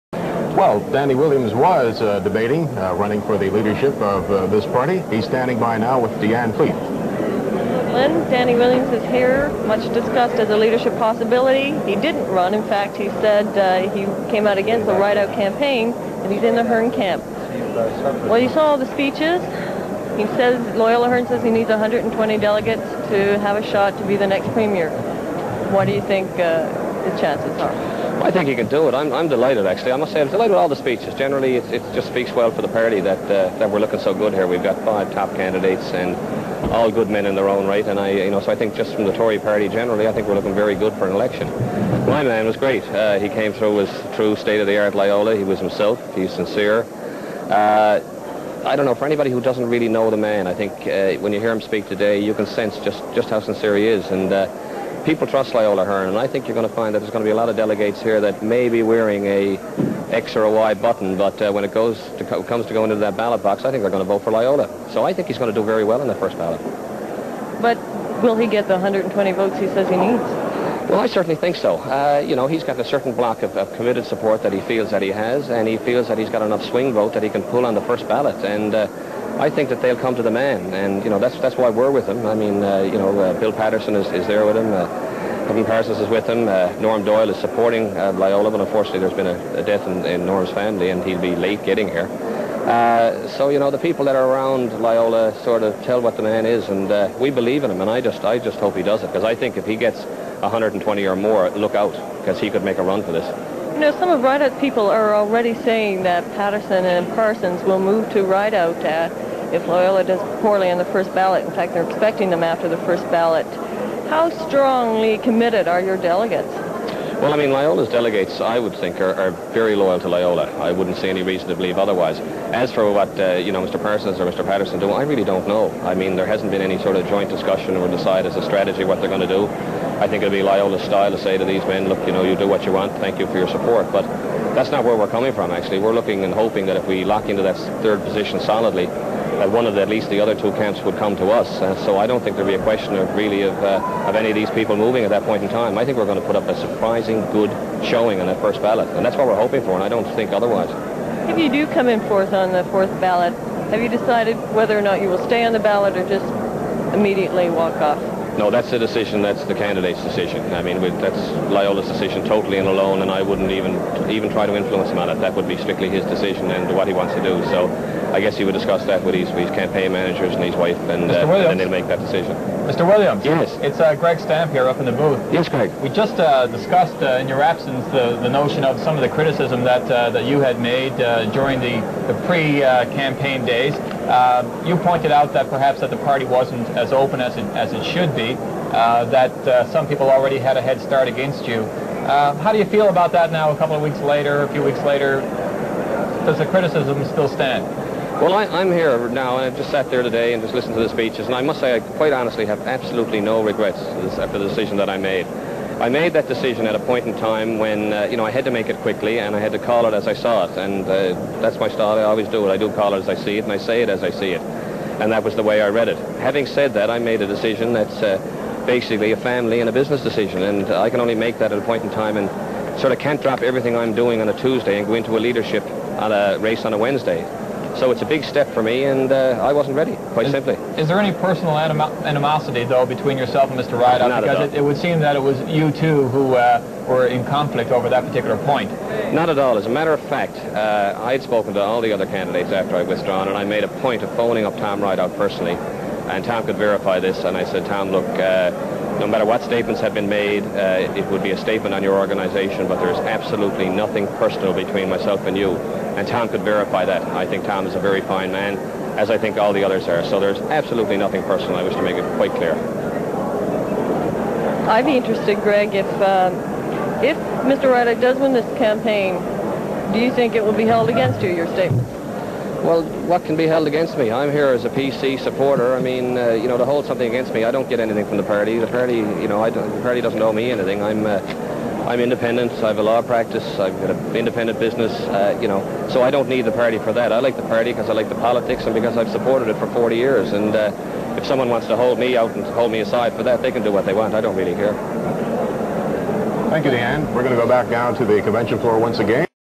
Danny Williams in 1989
Here's a clip of Danny Williams speaking to CBC reporter Deanne Fleet at the P.C. leadership convention on March 11, 1989.